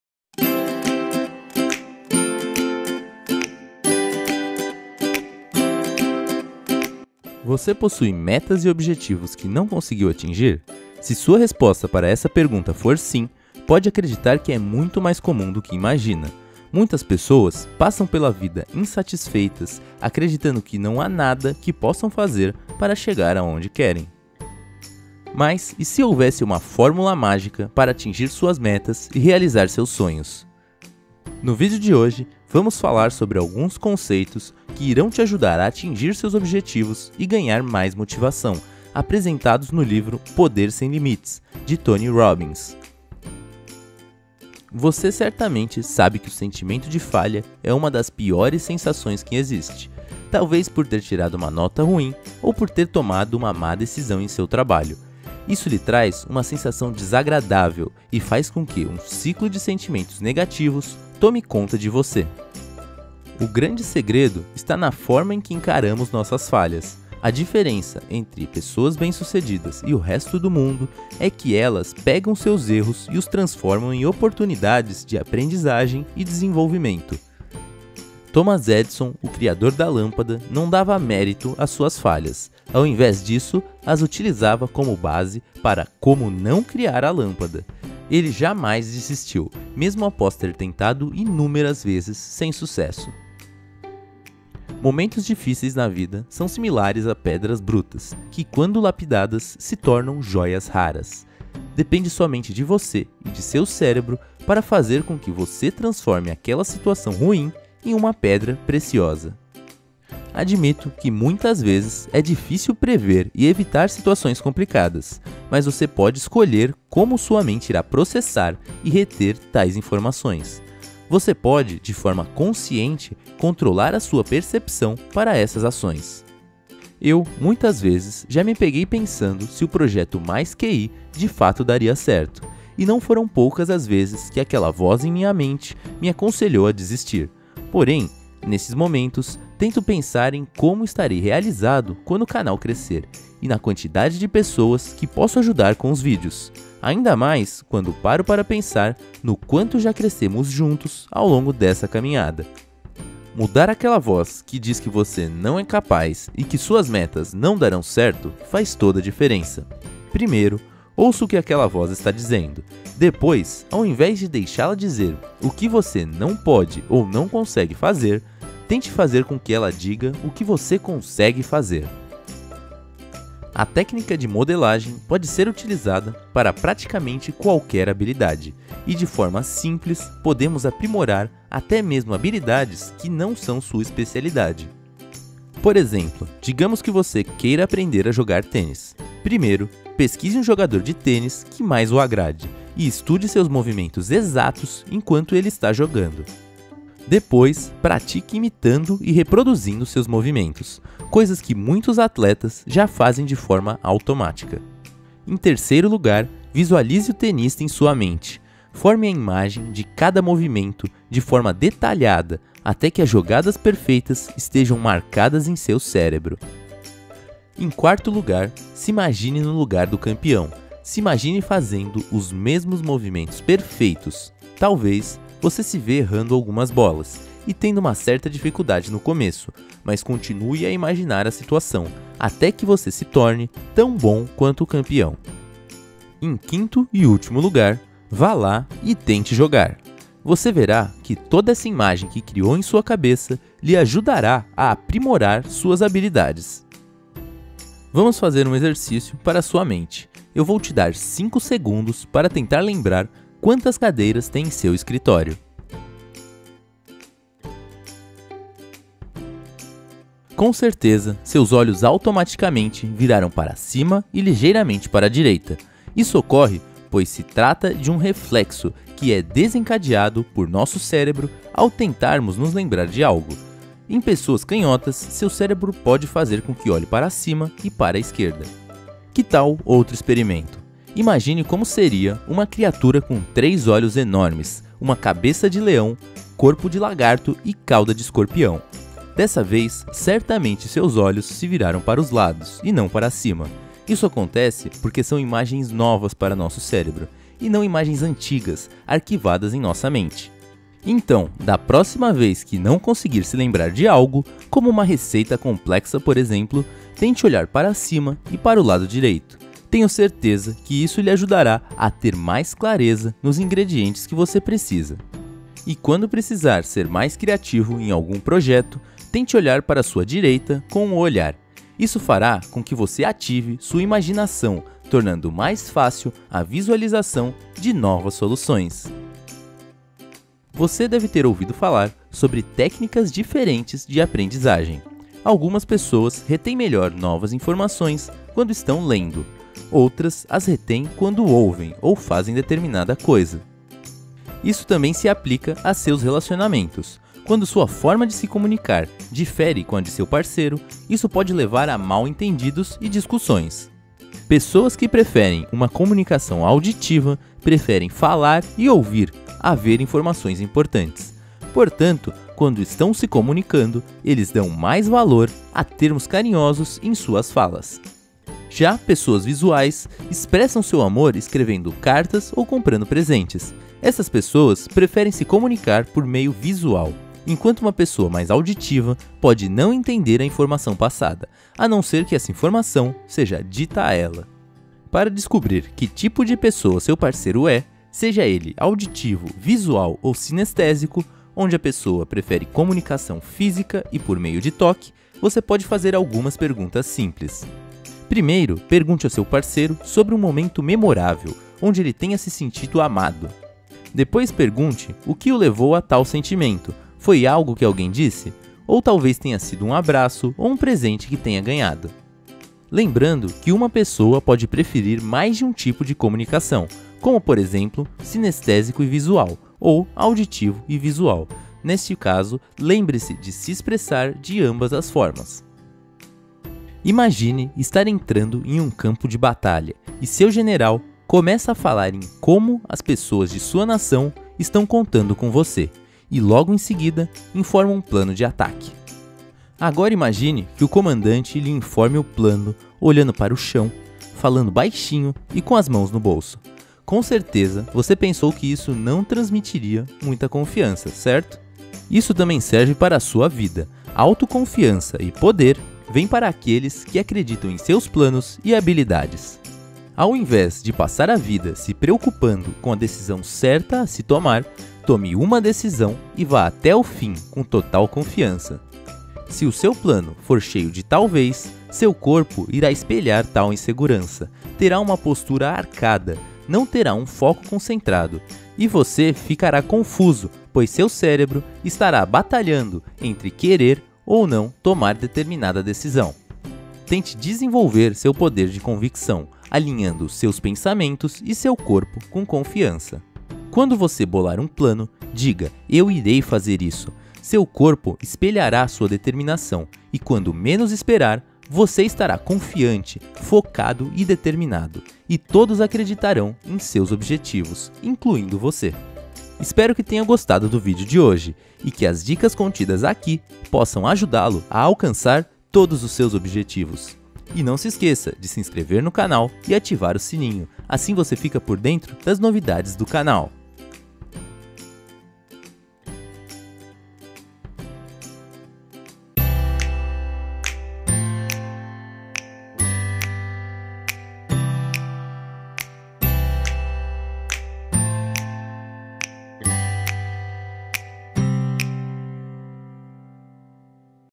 PODER SEM LIMITES - Anthony Robbins - Resumo animado
Descrição:

Você possui metas e objetivos que não conseguiu atingir? Se sua resposta para essa pergunta for sim, pode acreditar que é muito mais comum do que imagina. Muitas pessoas passam pela vida insatisfeitas, acreditando que não há nada que possam fazer para chegar aonde querem.
Mas e se houvesse uma formula mágica para atingir suas metas e realizar seus sonhos?
No vídeo de hoje, vamos falar sobre alguns conceitos que irão lhe ajudar a atingir seus objetivos e ganhar mais motivação apresentados no livro "Poder sem limites" de Tony Robbins.